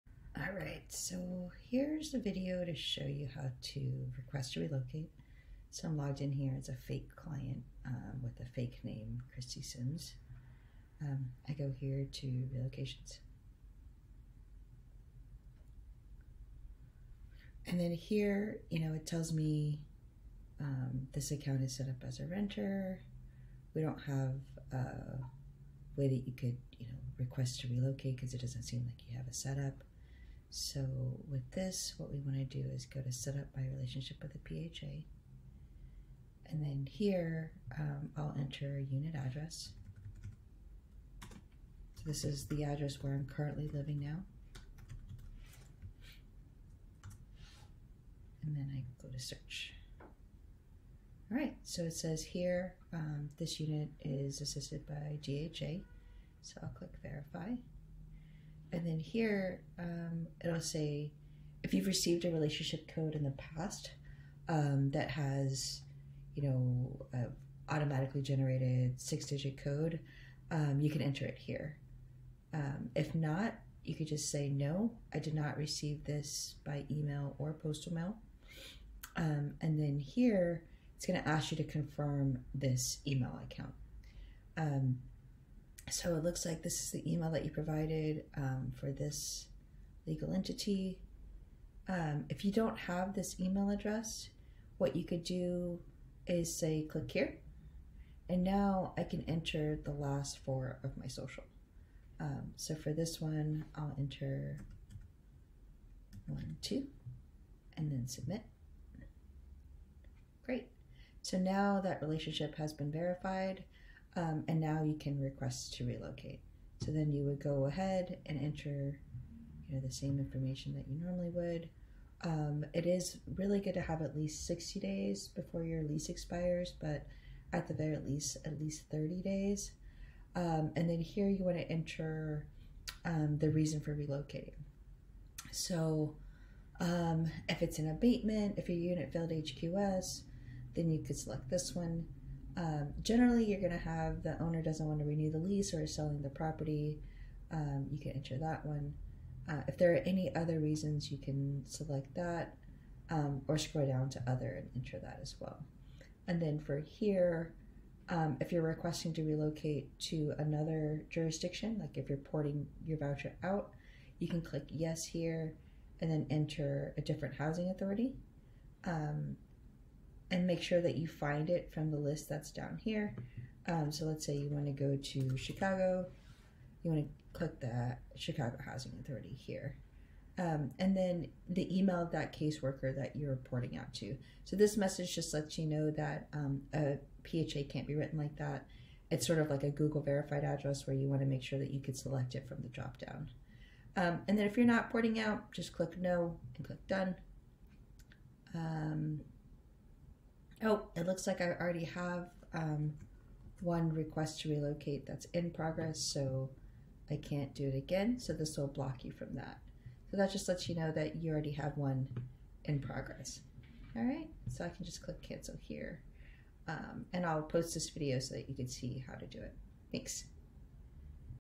Request to Relocate with no Relationship Code
Here's a quick video to show you how to request to relocate without a relationship code in Bob.ai